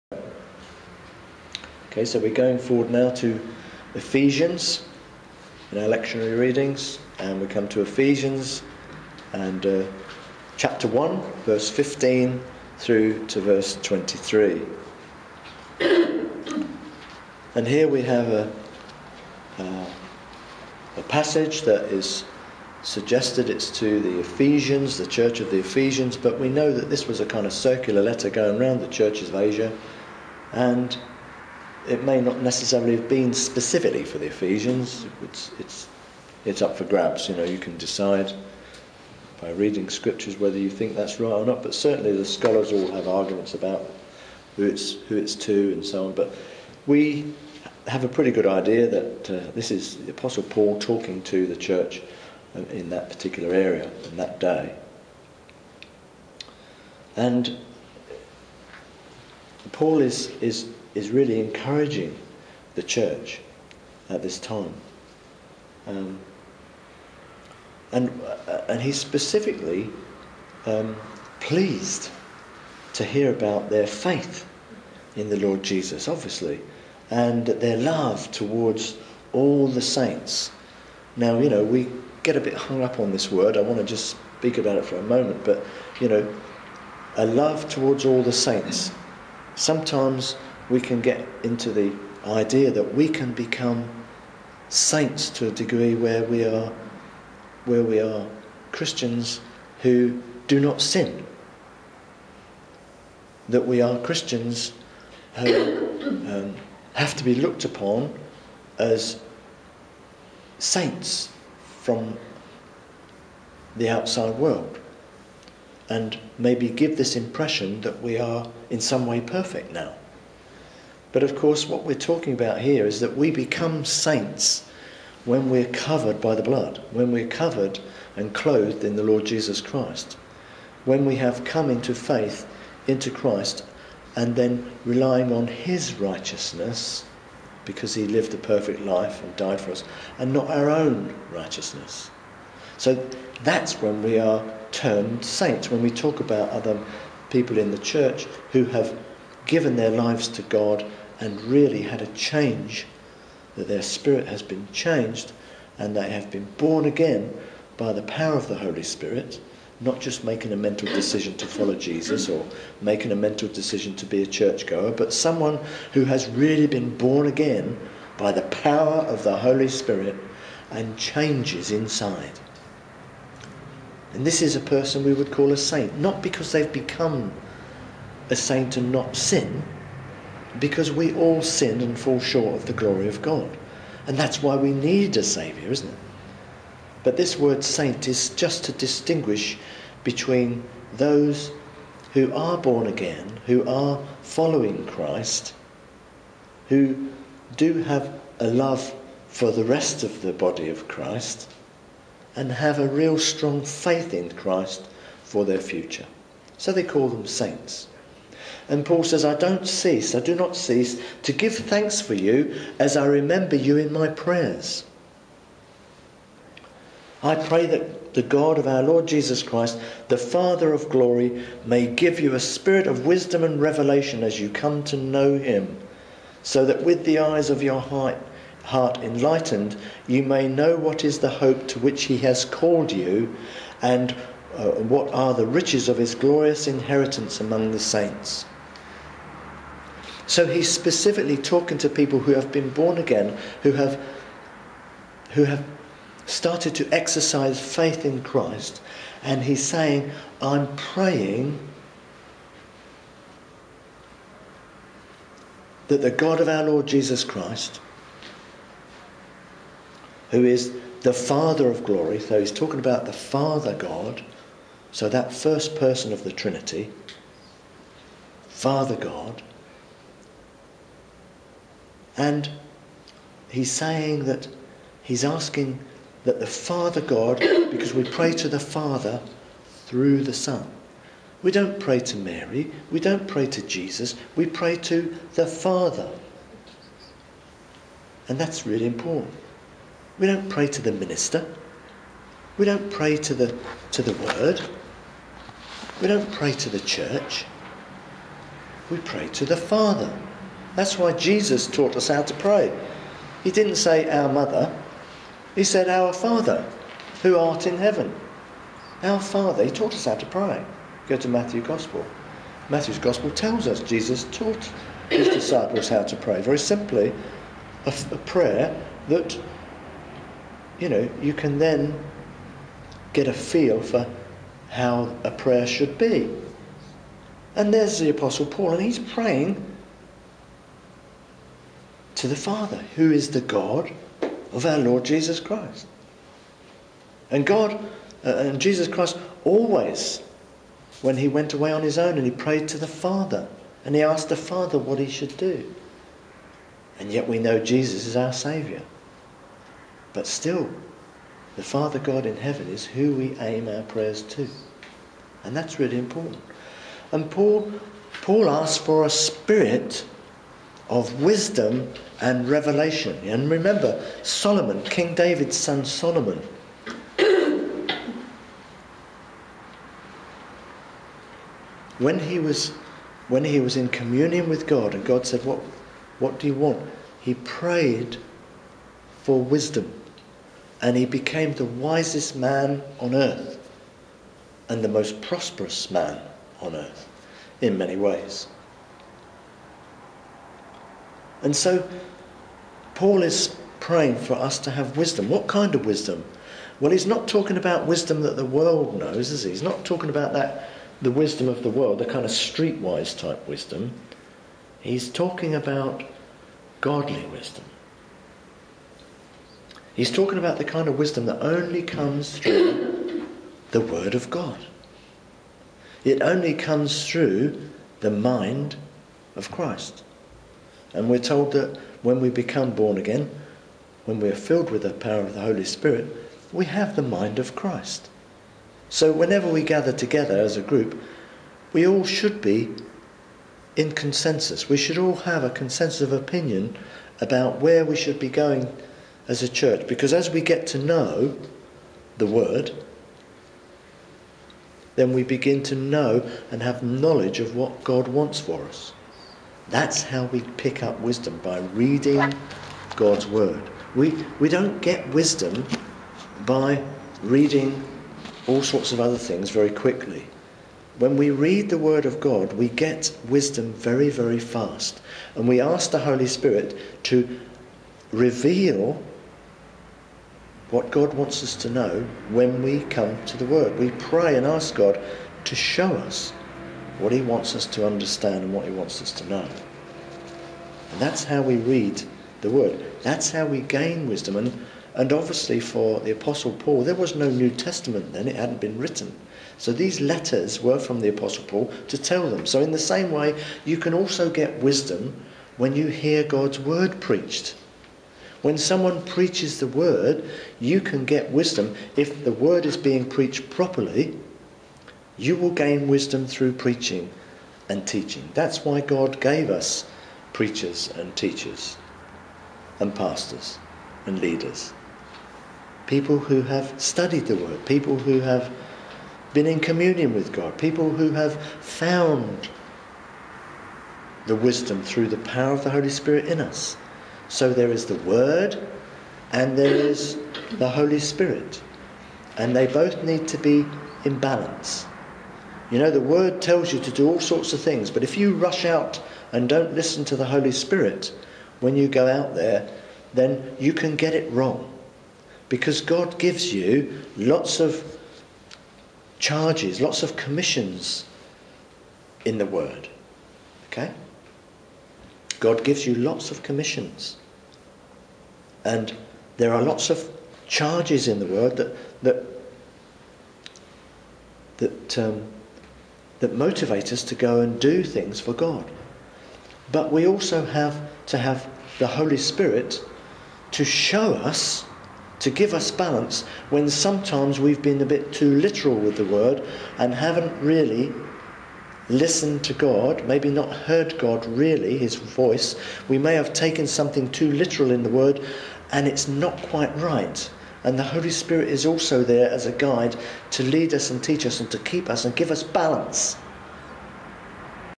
God's Flock 3 of 7 - Bishop George 20th Nov 11
Bishop Dr George Booty preaching from the New King James Bible. These are daily readings from the Anglican Lectionary at St Georges Church - ACORN HOUSE, in Central Milton Keynes. England. UK. MK9 3HP -- Normal 10am Service - 13h Nov 2011.